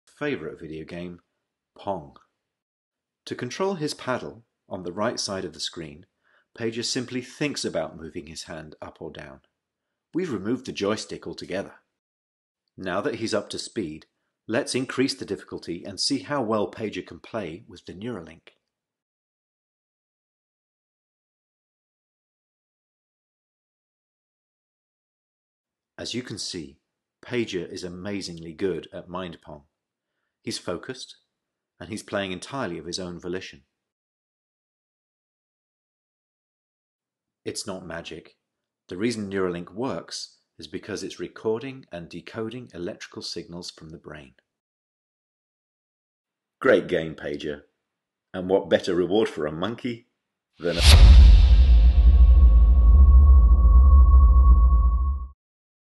Maymun, kelimenin tam anlamıyla bir beyin çipi kullanarak telepatik olarak bir video oyunu oynuyor!
“Bir maymun, kelimenin tam anlamıyla bir beyin çipi kullanarak telepatik olarak bir video oyunu oynuyor! Yakında maymununuz Twitch ve Discord’da olacak.”